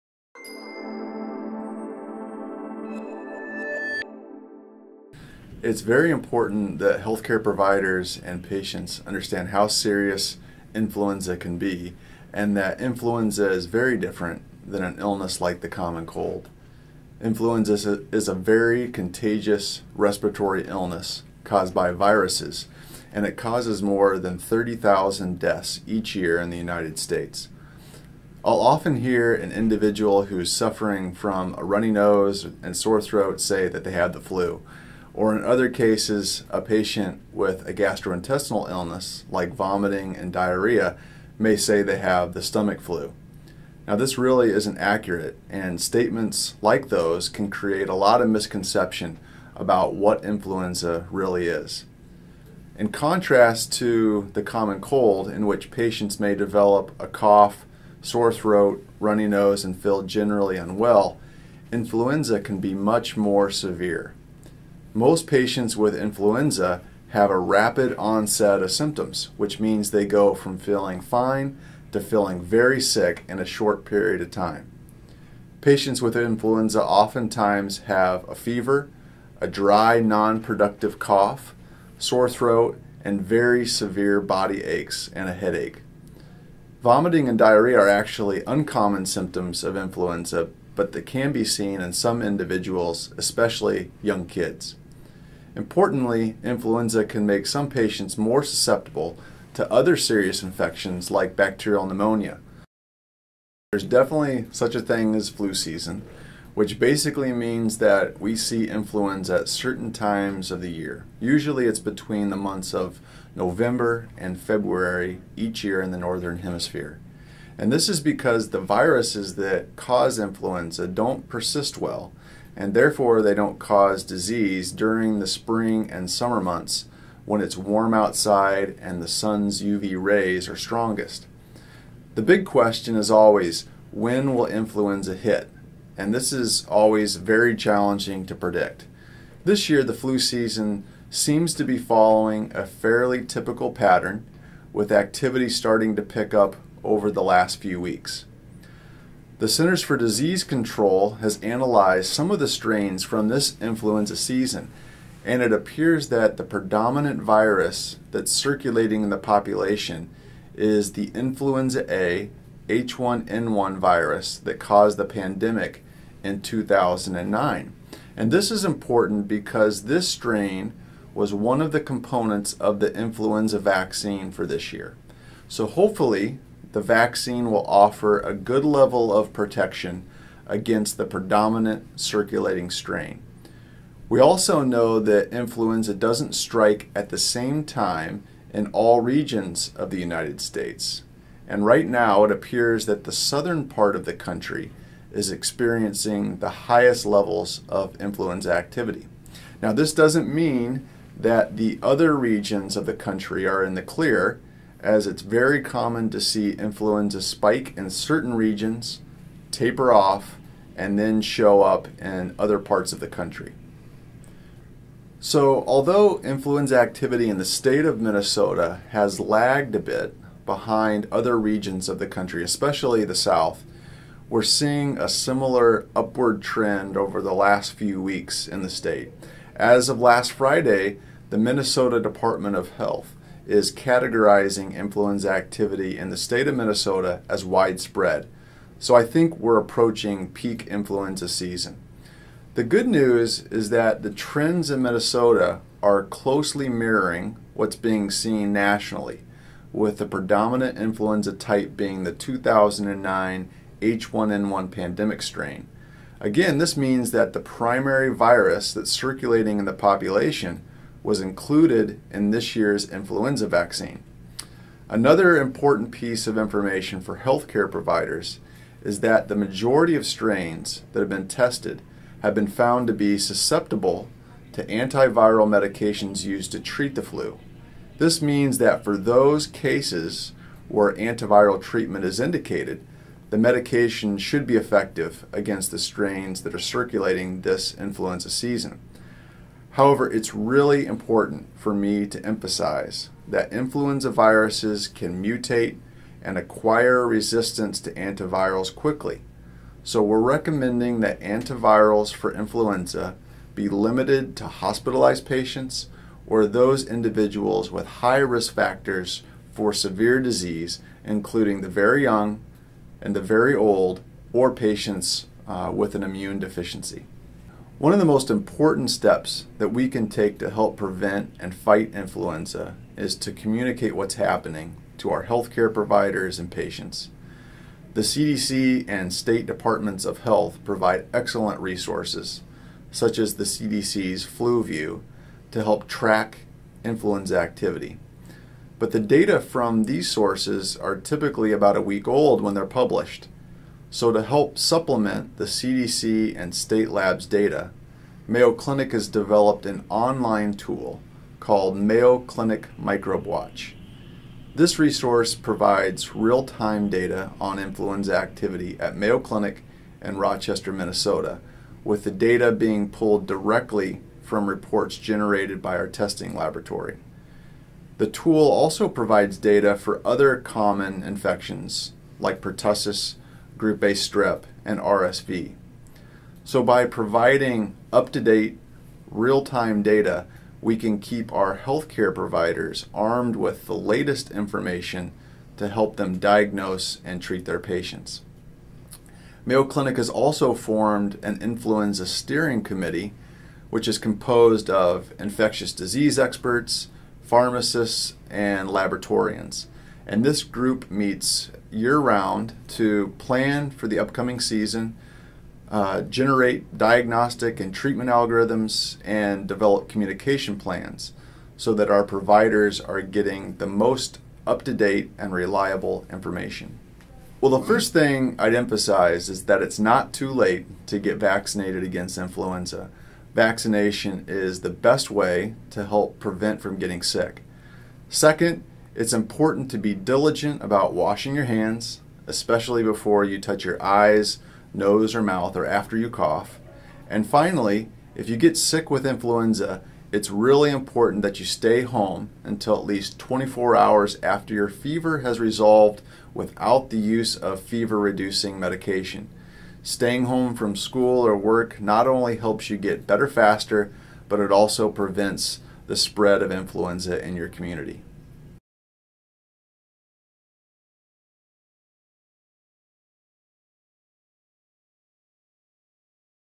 Mayo Clinic Microbe Watch Flu Update - Jan. 9
Mayo Clinic's Division of Microbiology will be posting weekly updates throughout this year's flu season. In this video, Dr. Matt Binnicker discusses the symptoms of the flu, the flu season, and what trends we are seeing nationally, in Minnesota and at Mayo Clinic in Rochester.